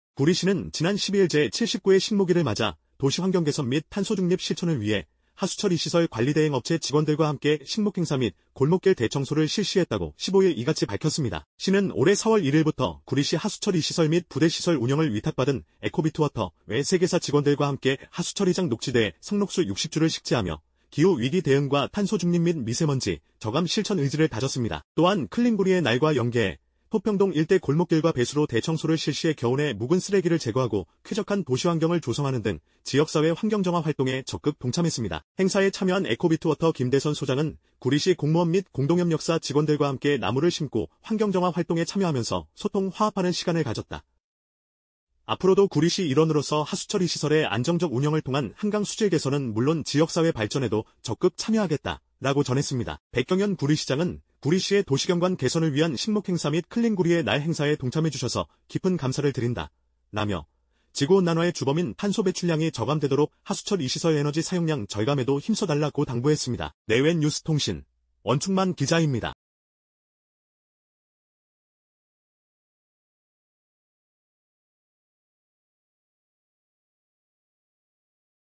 구리시, 하수처리시설 관리대행 직원들과 식목행사 개최
구리시(시장 백경현)는 지난 12일 제79회 식목일을 맞아 도시환경개선 및 탄소중립 실천을 위해 하수처리시설 관리대행 업체 직원들과 함께 식목행사 및 골목길 대청소를 실시했다고 15일 이같이 밝혔다.

취재 원충만 기자
편집 김현우 기자